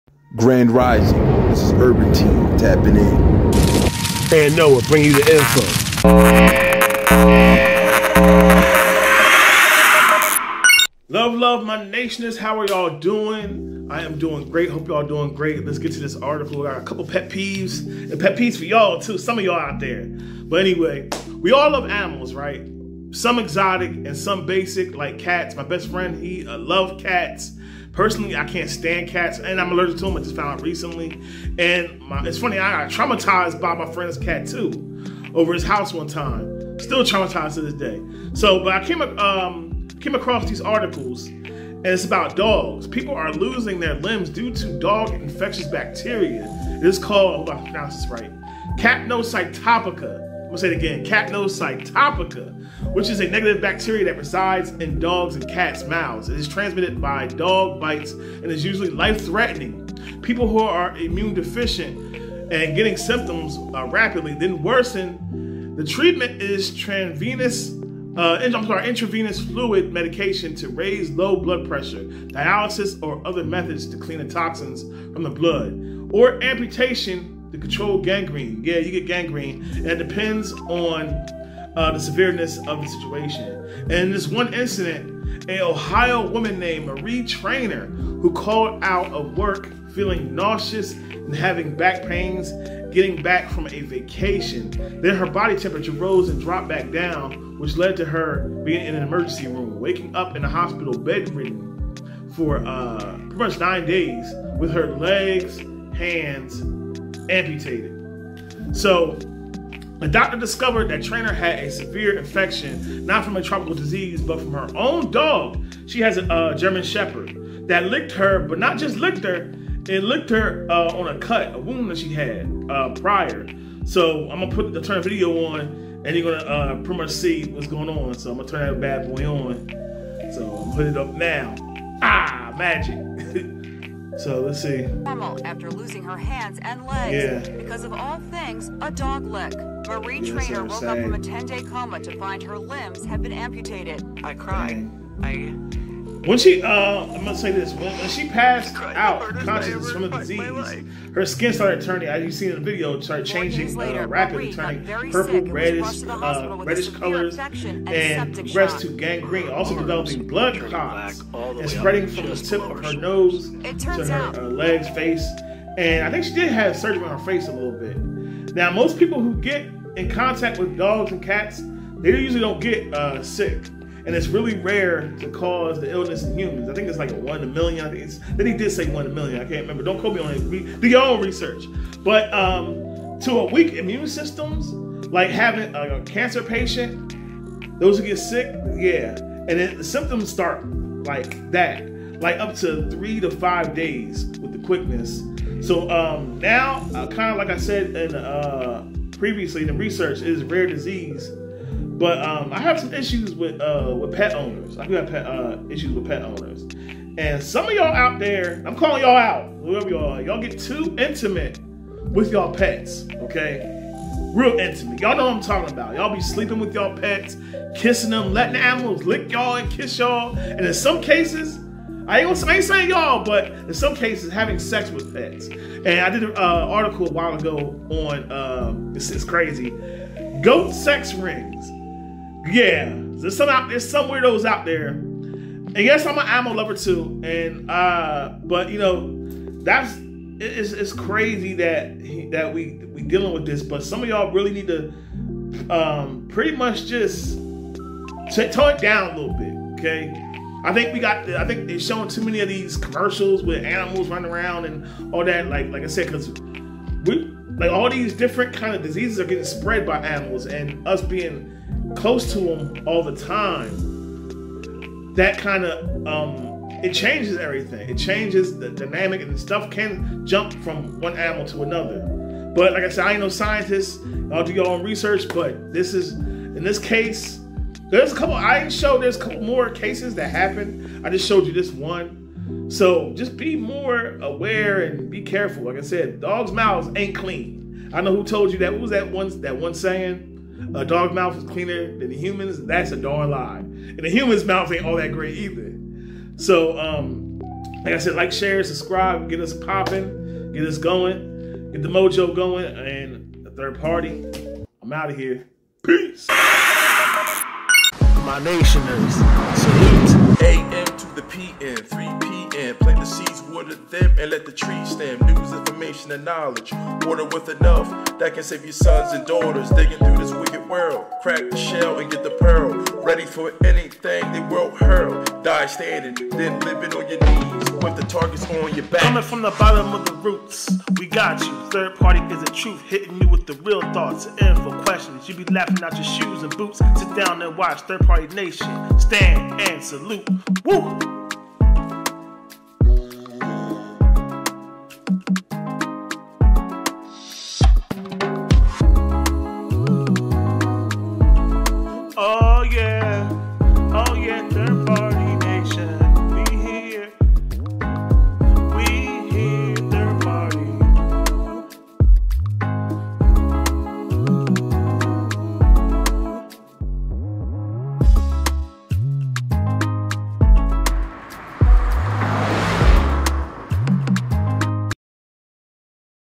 Are to Many People Getting Intimately Obsessed Over Dogs Also getting Affected by Capnocytophaga
We all love  animals some exotic and some basic like cats  my best friend love cats personally I can't stand cats and I'm allergic to them had a friend's cat attack me once  trauma anyway and most people's dogs butts doing some research I came across a couple of articles with   People losing their limbs Due To a Dog infectious bacteria

It's called CAPNOCYTOPHAGA
Which is a negative bacteria that resides in dogs and cat's mouths is transmitted by dog bites and is usually life-threatening The people who have immunodeficiency The symptoms worsen rapidly  treatmentIntravenous fluids, Medications to raise low blood pressure, Dialysis or other methods to clean toxins from the blood, and Debridement or amputation to control gangrene. That's depending on severeness of the situation

And this one incident is a Ohio woman named Maria Trainer who called out of work feeling  nauseous and also her back hurting just getting back from a vacation  then her temperature  Rose drop back down This which let her to the emergency room waking up in the hospital bed 9 days later her legs and hands have been amputated

The doctors discovered that trainer had a severe infection Not from a tropical disease But from her own dog a German Shepherd that licked her But not just licked her lick her on a cut that she got

When she passed out to my consciousness from a disease her skin start changing rapidly turning purple red color then progress to gangrene also developing blood clots spreading from the tip of her nose ears legs face she didn't lose parts of her face

Now most people who get in contact with dogs and cats don't get sick it's really rare to cause illness and humans But with a weekend immune system just like cancer patience so those who get sick what's your symptoms in 3 to 5 days or earlier

Now kind of like I said in the research it's a rare disease to get but I have an issue with a lot of people and their pets They are overly intimate with their pets

Sleeping with a pets kissing them letting the animals kiss them And there's some out there even having sex with them let's be real I did an episode a while ago on a goat sex ring But I'm not going there

And yes I'm an animal lover too but come on guys That's why a lot of these diseases are spreading too humans because we do too much we looking more for companionship in animals than actually human This might hit some people in their chest stop letting animals lick your mouth right That's all I'm saying lol